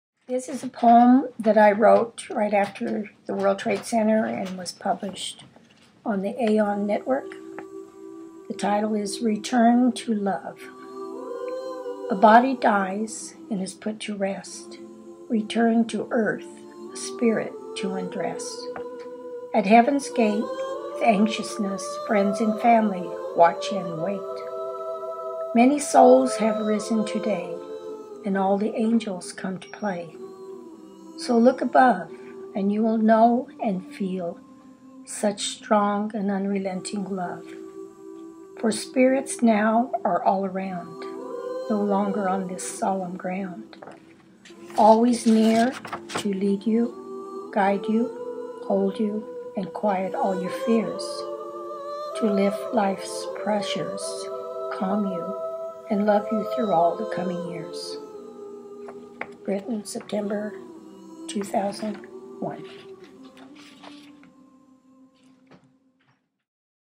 Return To Love Margaret Davis - Haley
I wrote this poem after 911, it was published on AON's website.
September 2001